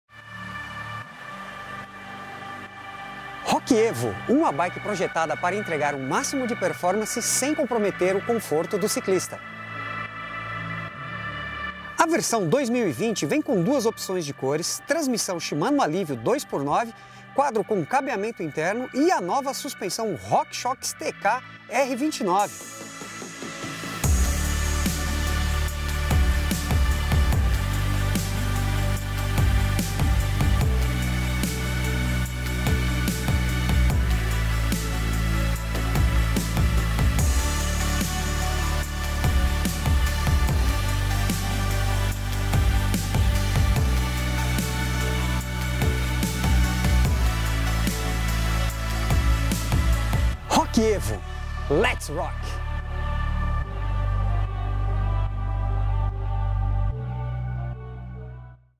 Making Sense 2020 - Sense Bike Rock Evo - Mountain Bike Hard Tail
Uma bike projetada para entregar o máximo de performance, qualidade e custo benefício incomparável. A versão 2020 vem com 2 opções de cores, transmissão Shimano Alívio 2×9, quadro com cabeamento interno e geometria esportiva, e vem agora com a nova suspensão ROCK SHOX TK R29. Let’s Rock!